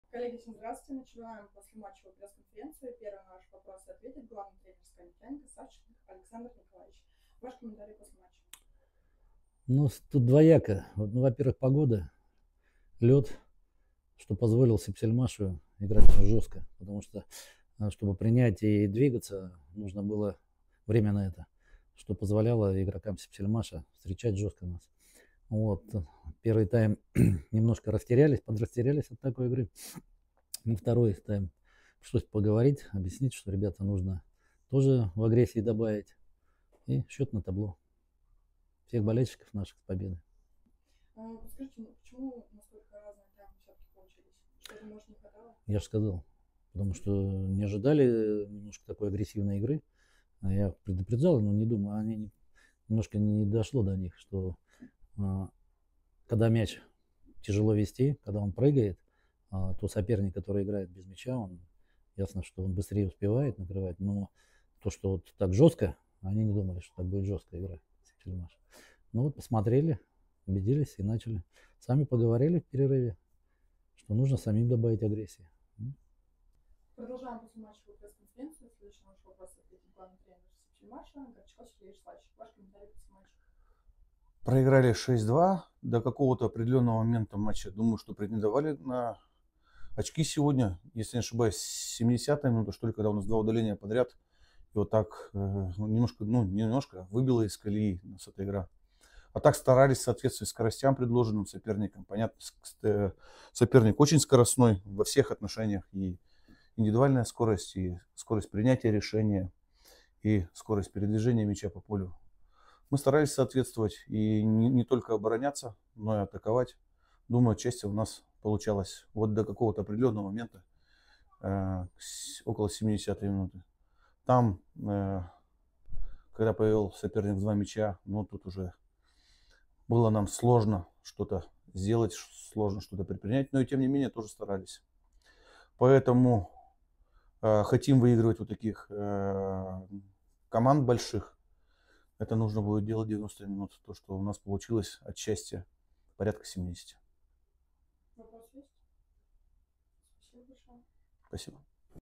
Пресс-конференция «Сибсельмаш» — «СКА-Нефтяник» (2:6)
Горчаков Сергей и Савченко Александр
прокомментировали сегодняшний матч